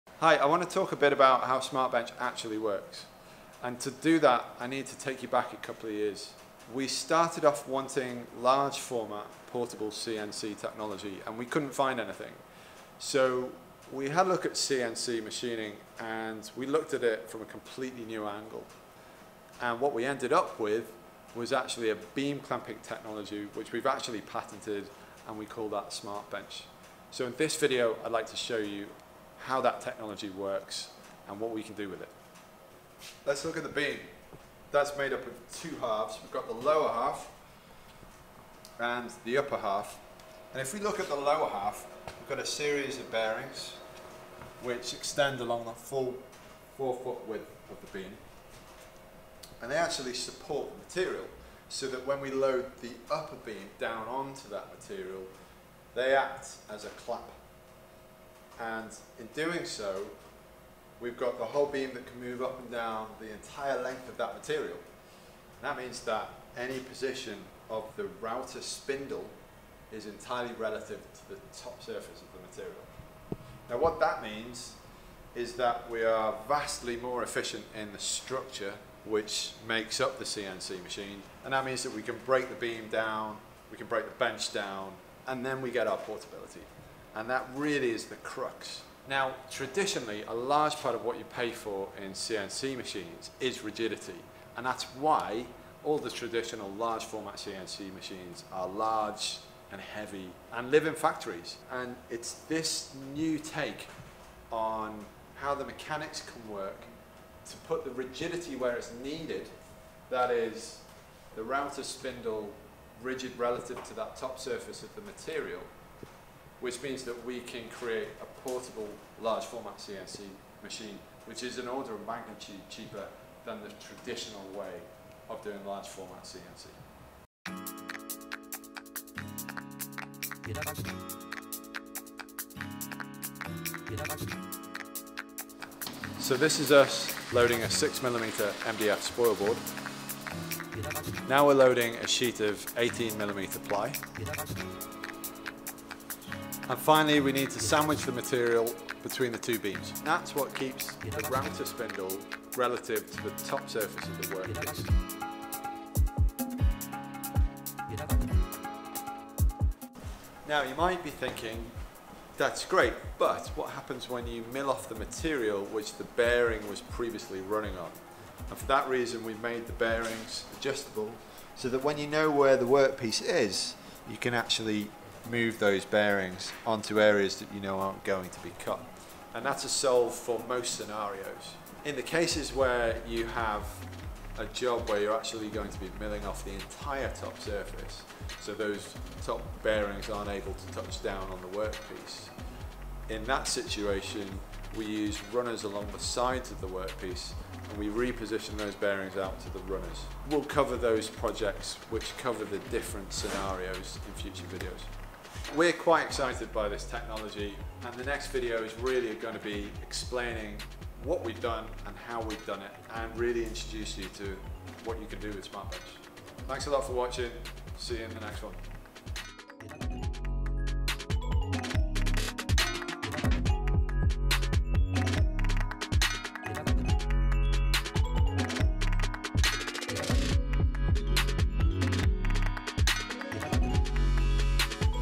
SmartBench: Beam Clamping Technology
**This video uses one of the earlier versions of SmartBench. Features will differ from the product that is currently on sale from our website. (It's cool to see how far it's come though, right?)**

In this video Ed explains just how SmartBench is able to be both lightweight and portable, yet rigid enough in operation to achieve fast, accurate cuts.